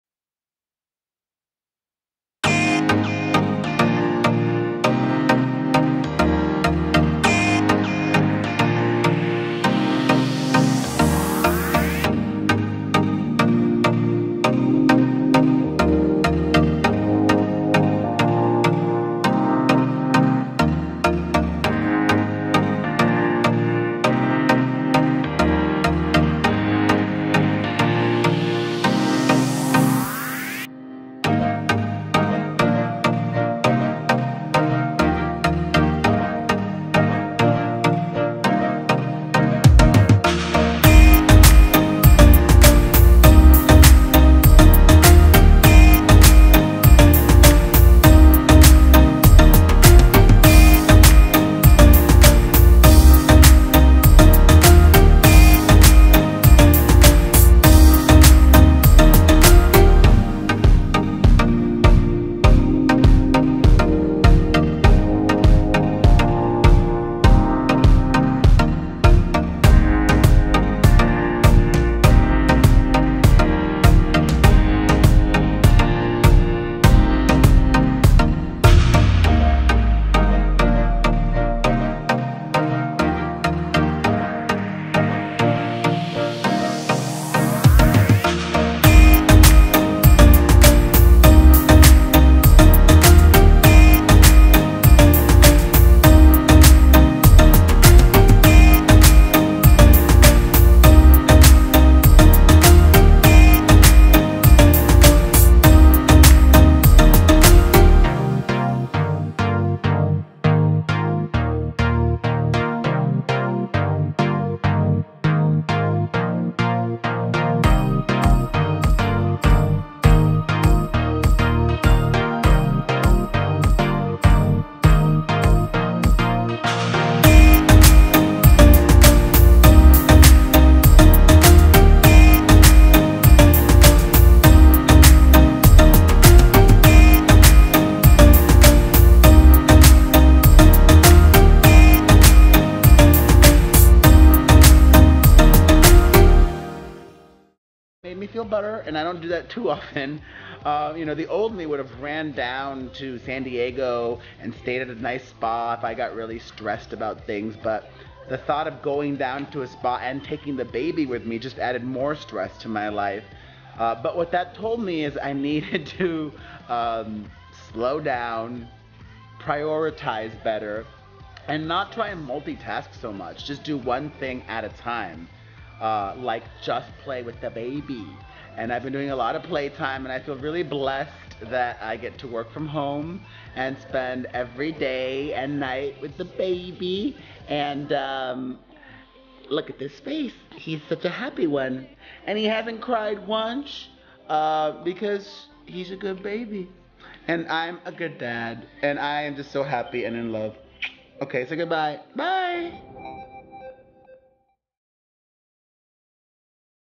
Checking In With Perez & Perez Jr.! | Perez Hilton
Poppa Perez vlogs about life as a new dad!

Check it out above!